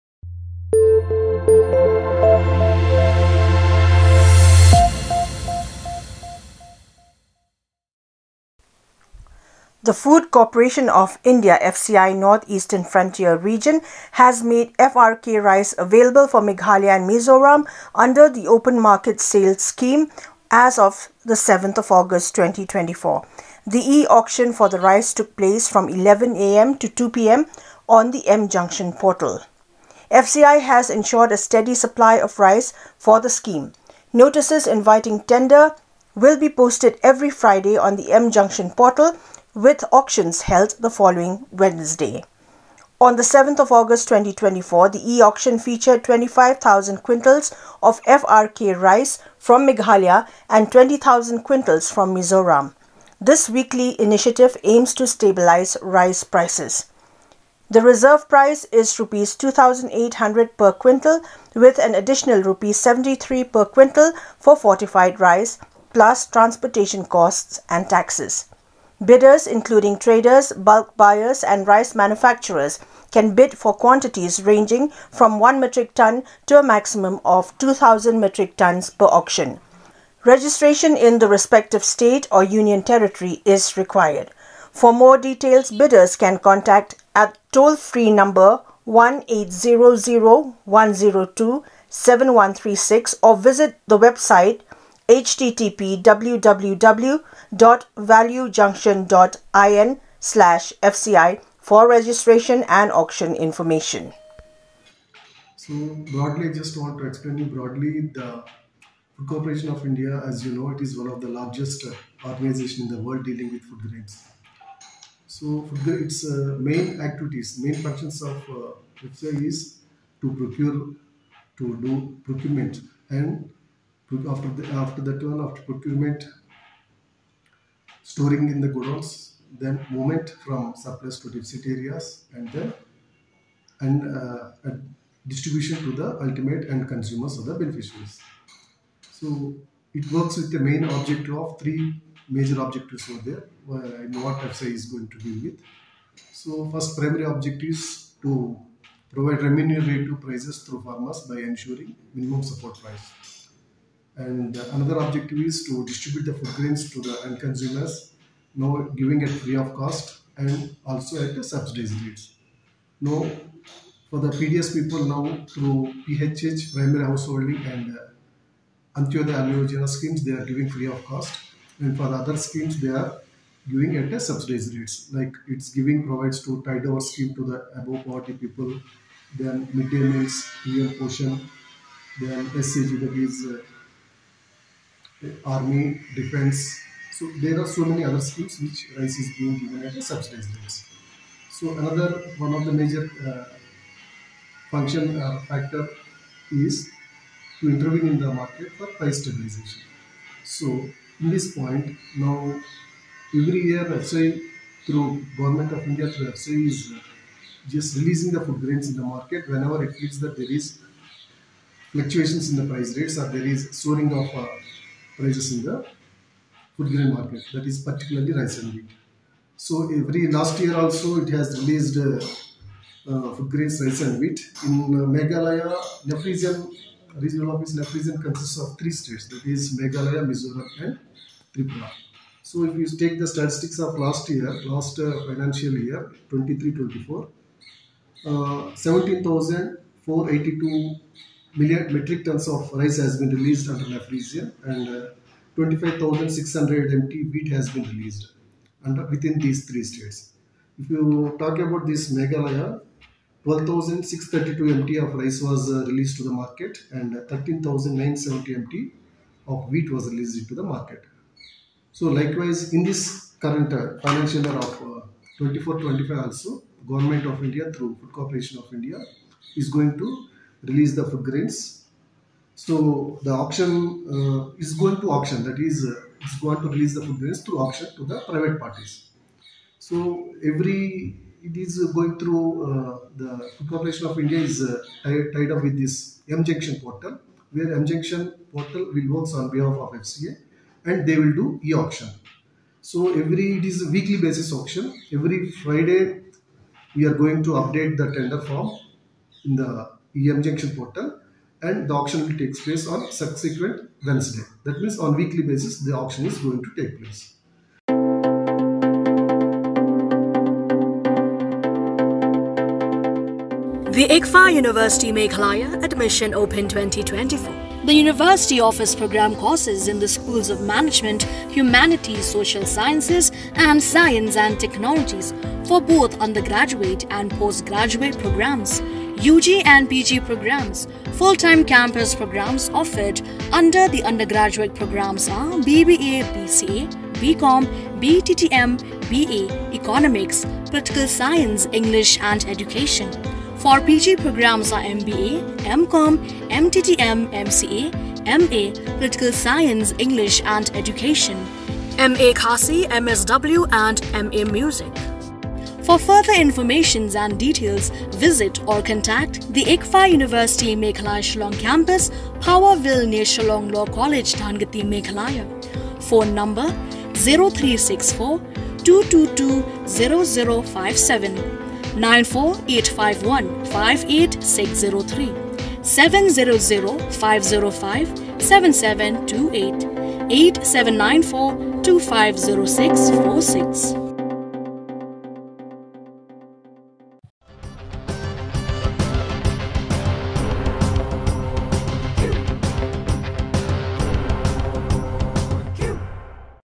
FCI LAUNCHES FRK RICE AUCTION FOR MEGHALAYA AND MIZORAM ON AUGUST 7, 2024
The Food Corporation of India (FCI) North Eastern Frontier Region has made FRK rice available for Meghalaya and Mizoram under the Open Market Sale Scheme (OMSS-D) as of August 7, 2024. The e-auction for the rice took place from 11 a.m. to 2 p.m. on the MJUNCTION portal.